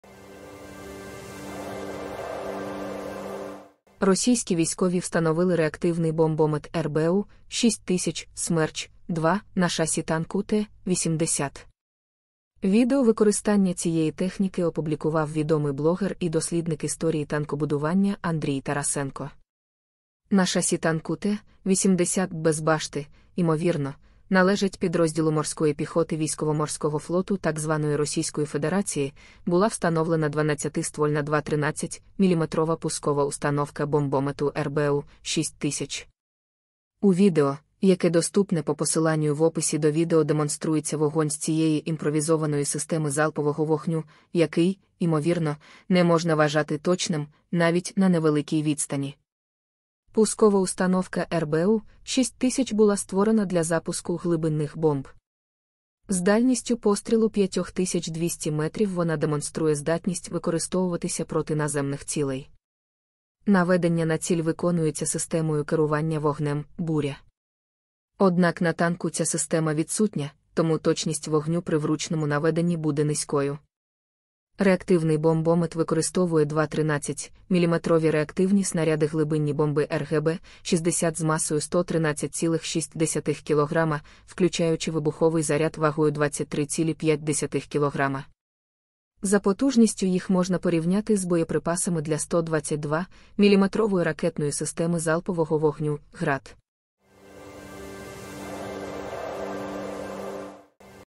"Ноу-хау" від росіян: корабельний 213-мм бомбомет замість башти танка Т-80
USDT: TSJfcHW5UBth2qsR6jYUeGsxJDYYL16YCL

Bitcoin
BTC: 1PYFjXGCBvzyjpbuPRiy1P7DwndWNcTKfG